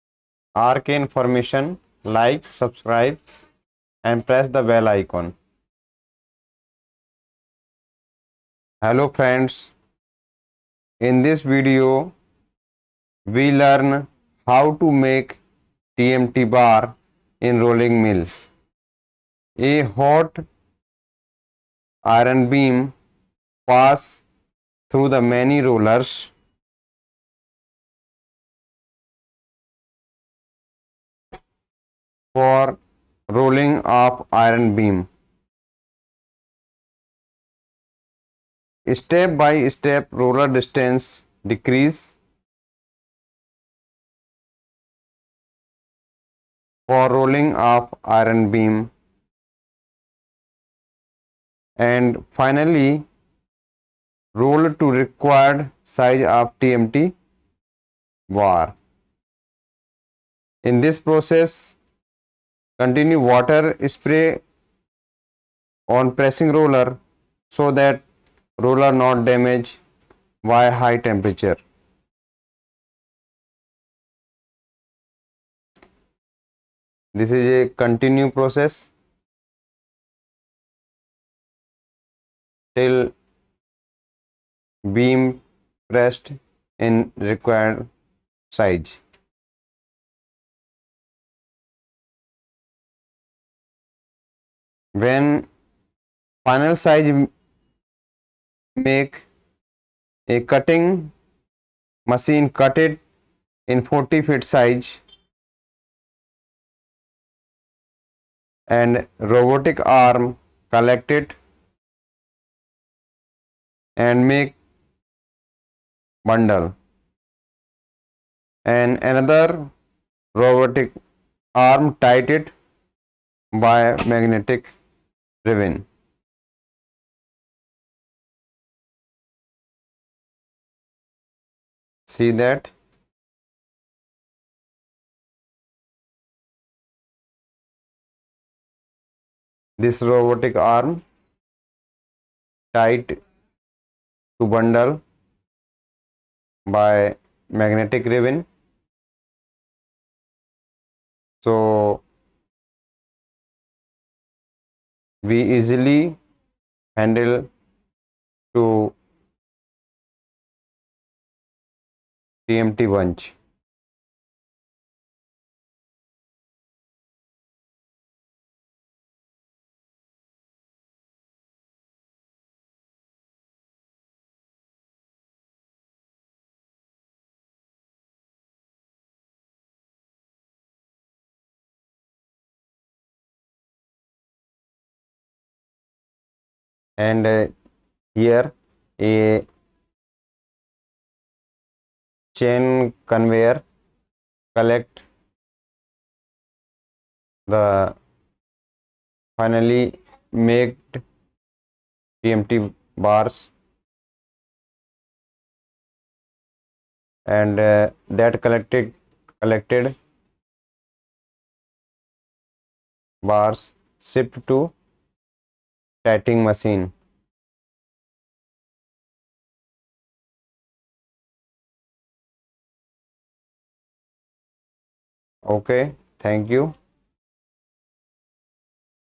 how to make TMT bar in rolling mill ,सरिये का निर्माण कैसे होता है,टी एम टी बार कैसे बनता है
in this video we learned how to make TMT bar